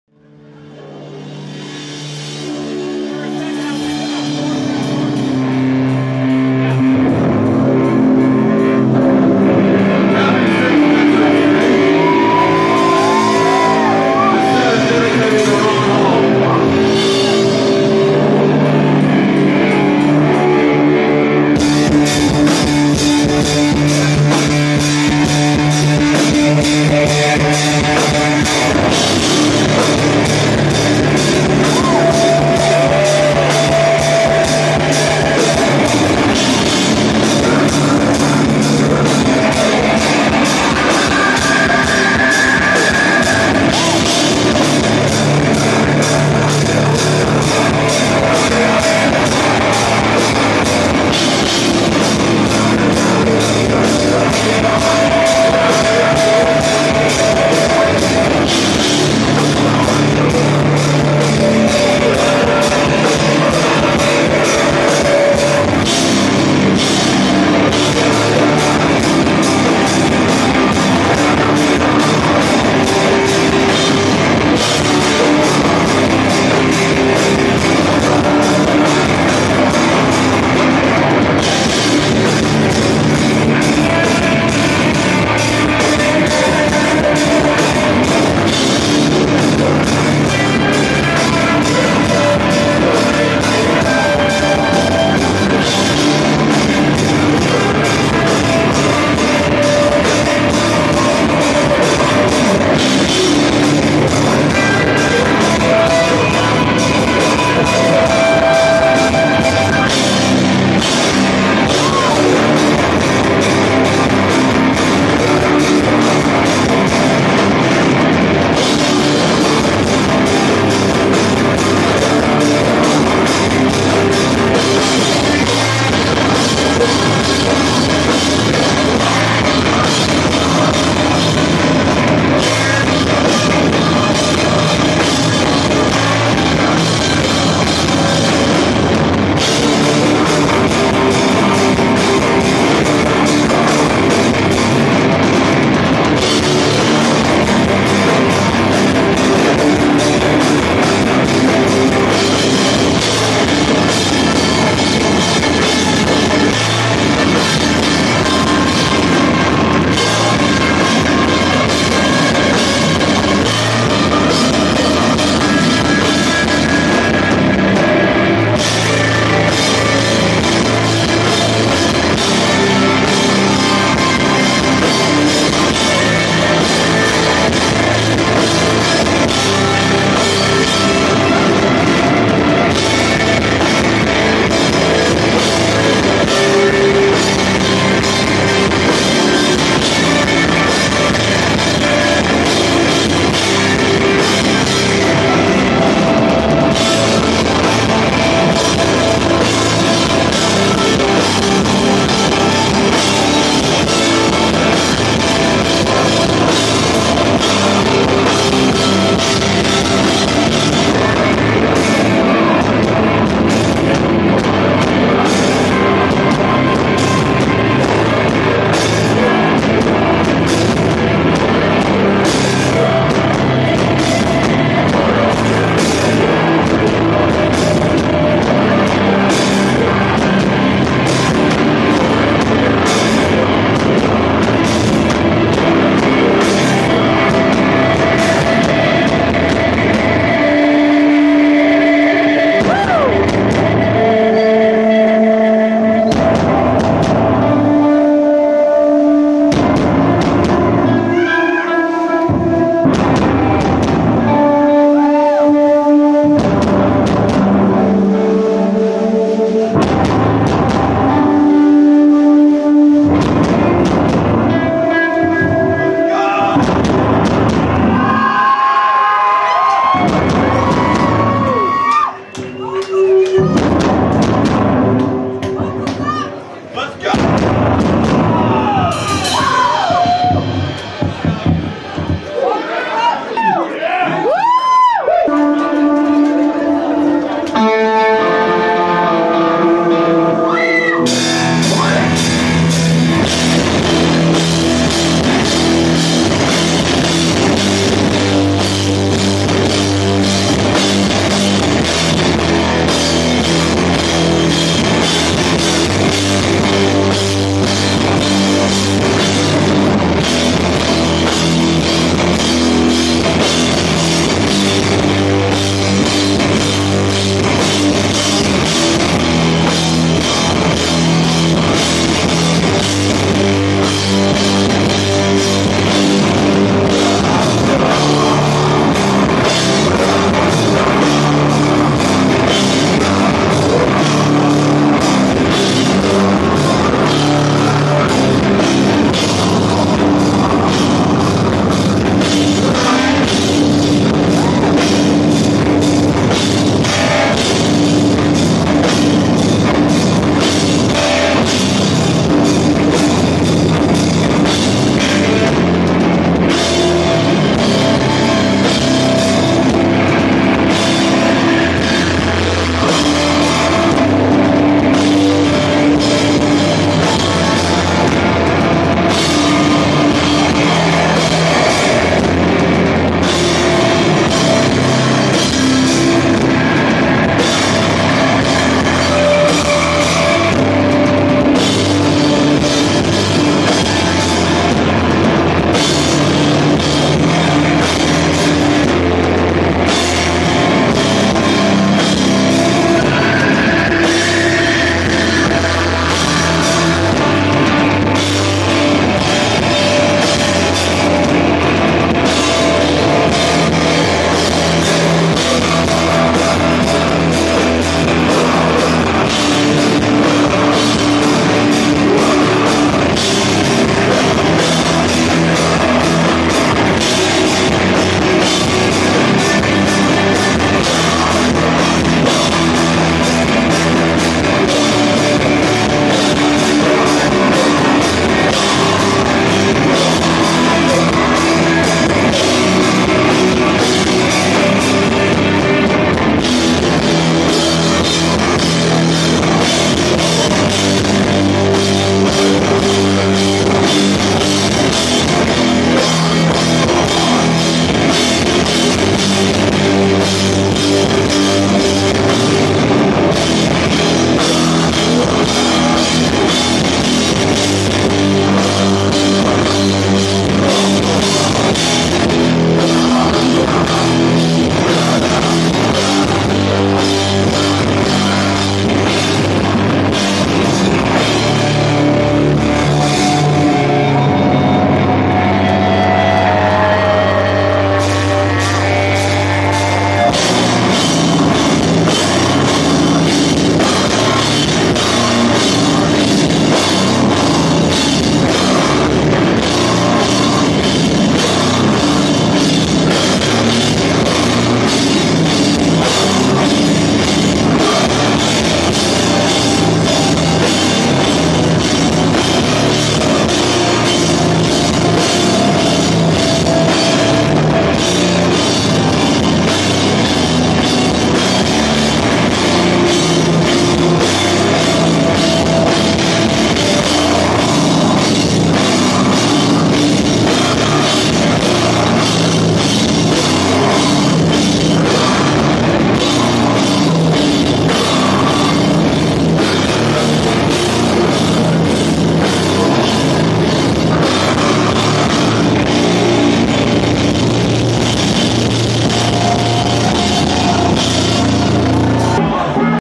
NIGHTS LIKE THESE Live at The Pyramid Scheme 2024
HEAVY. These guys drove up from Memphis to play this sick show and they really brought it down, one of my favorite sets I've seen in a long time and I hope we can get them back up here again before too long. Visuals by SUPER NUCLEAR